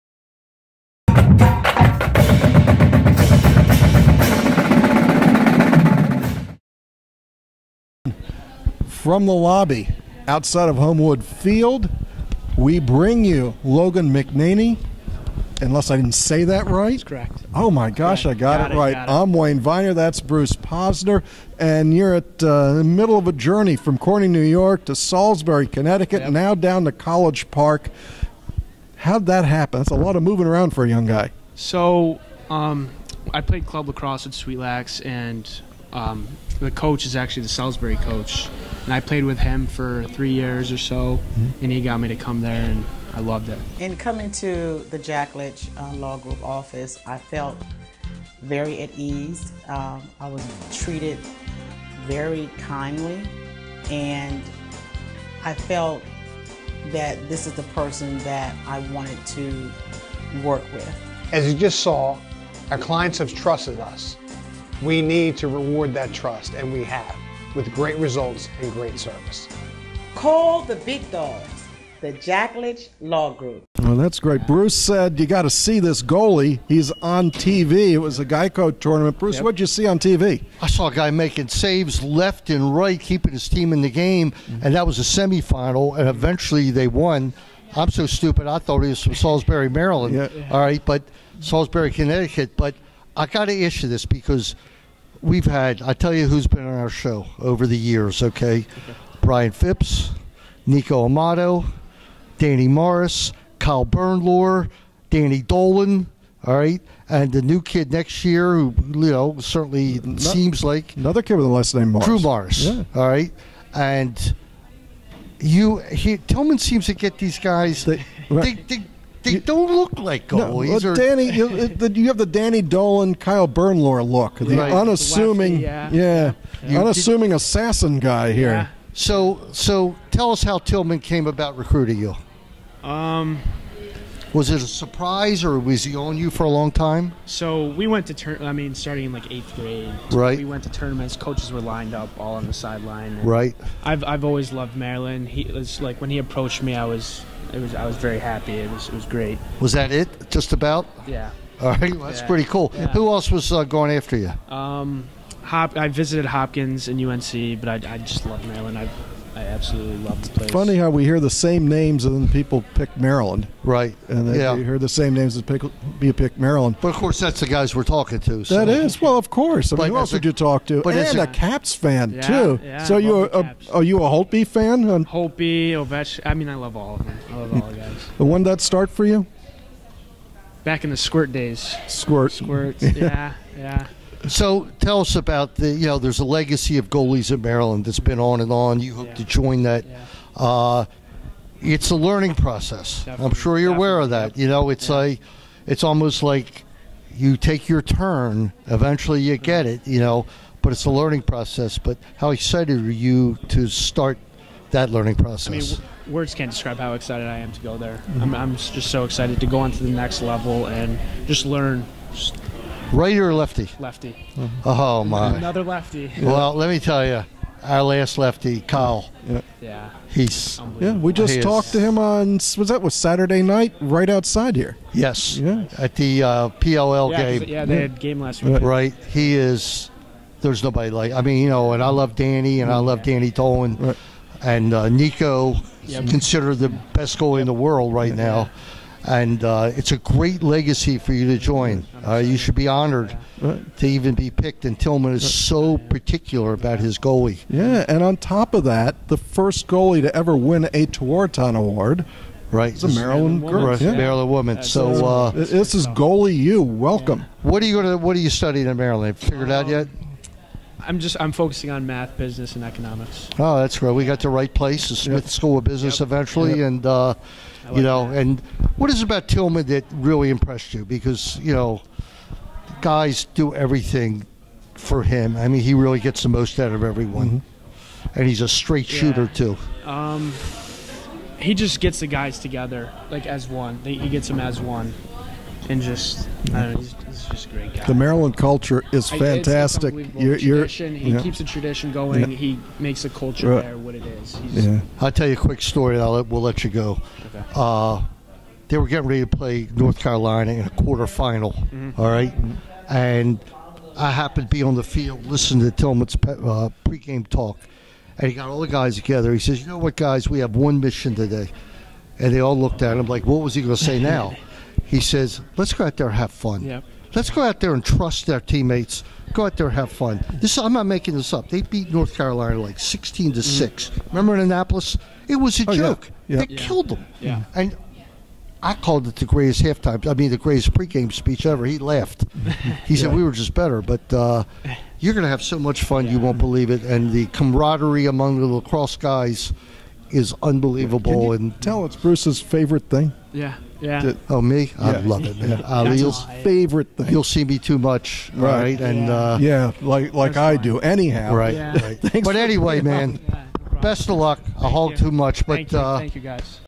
Lacrosse Under Armour All American Game 2019: Logan McNaney
Terp commit Logan McNaney talks Terps, caps and much more!